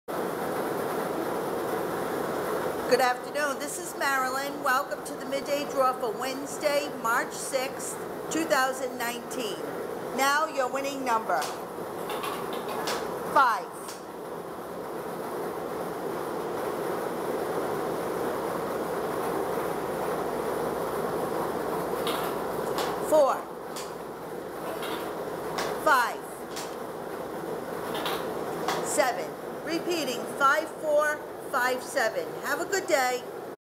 RI Lottery Midday Drawing 03/06/2019
Rhode Island Lottery's Midday Drawing for The Numbers on March 6, 2019.